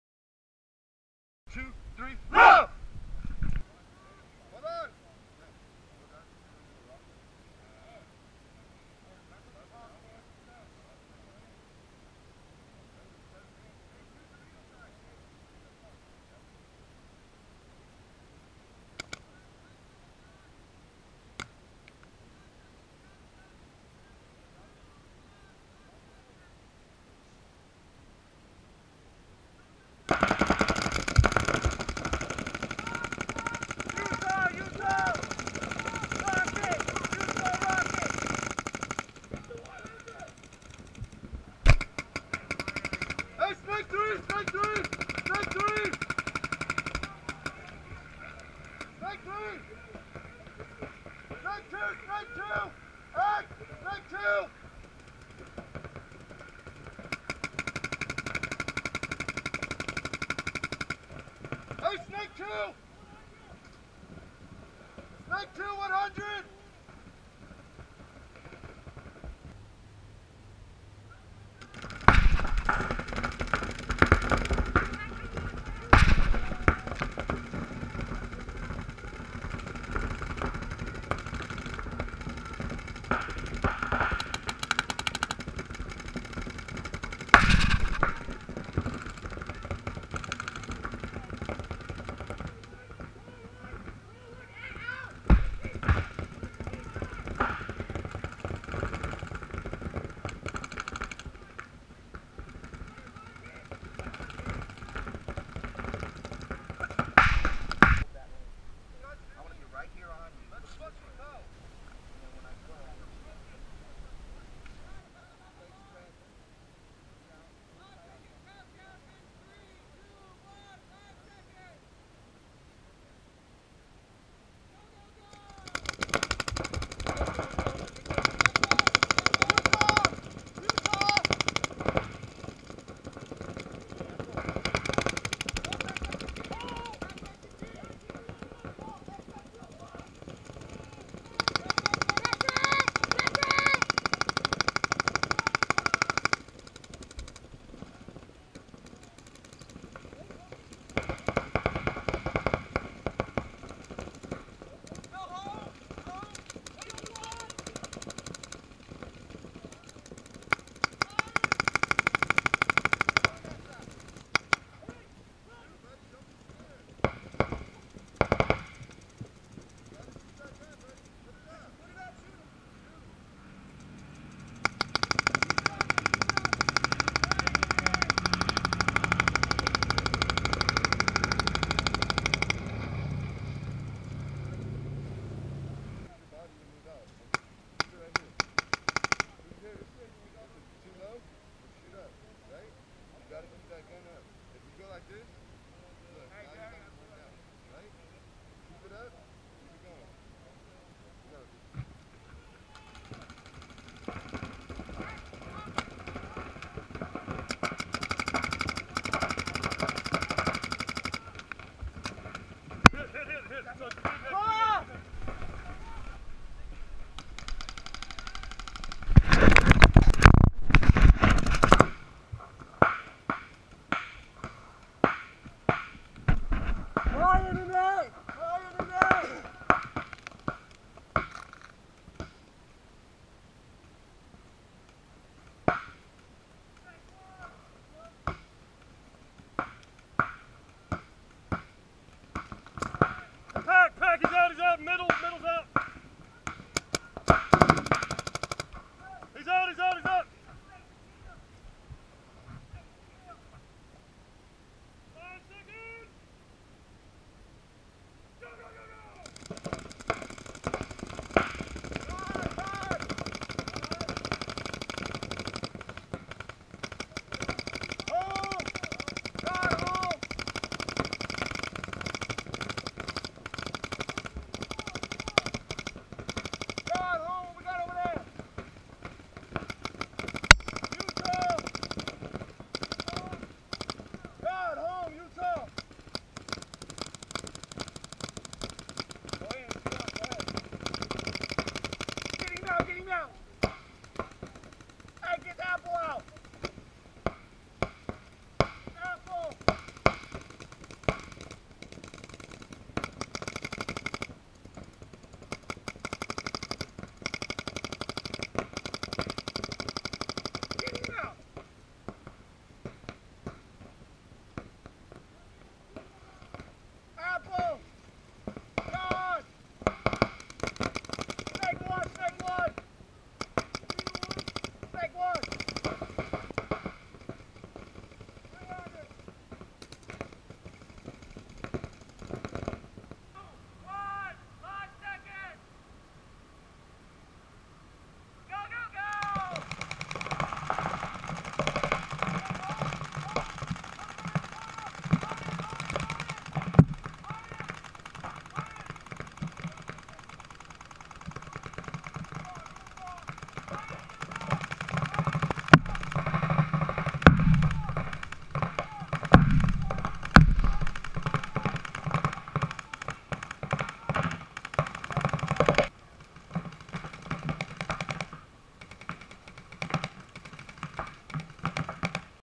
Entire NorCal Disruption Practice @ Midway Paintball Facility (Sunday, March 19, 2017)
NorCal Disruption - one of the teams we sponsor at NorCal Paintball in Vacaville, California - was at Midway Paintball Facility in Vacaville on Sunday to get ready for the WCPPL Las Vegas event in a couple of weeks.

Thanks!!